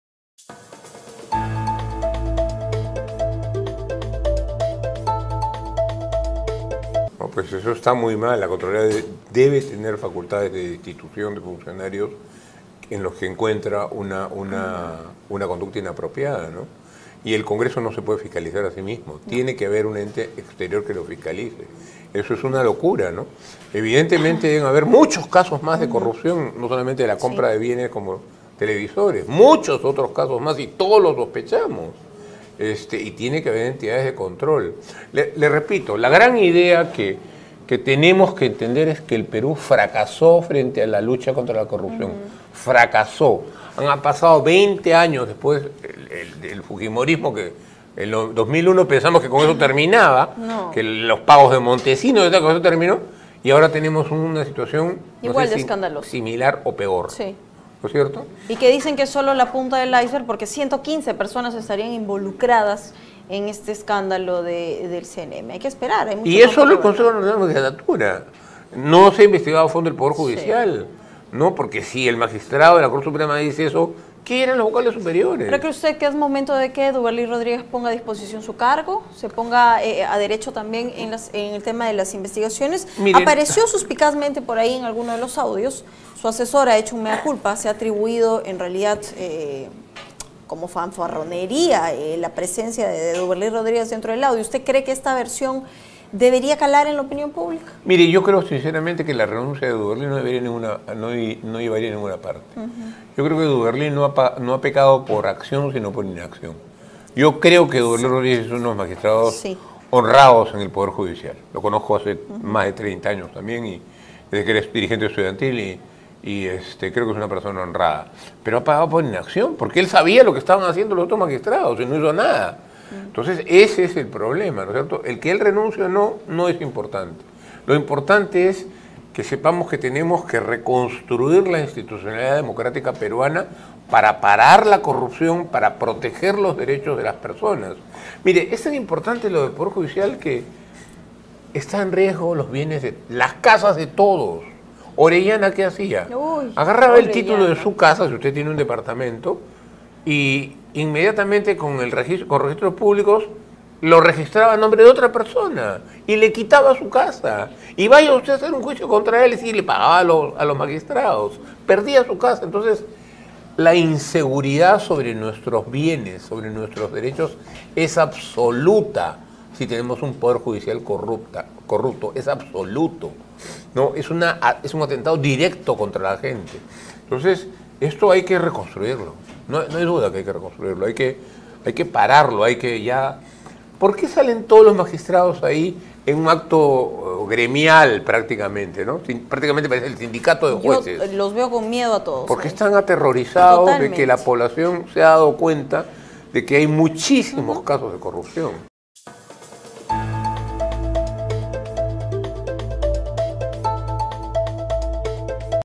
"El congreso no se puede autofiscalizar debe haber muchos más casos de corrupción", Carlos Chipoco.
[EntrevistaDigital]"El congreso no se puede autofiscalizar debe haber muchos más casos de corrupción", dijo Cralos Chipoco - Constitucionalista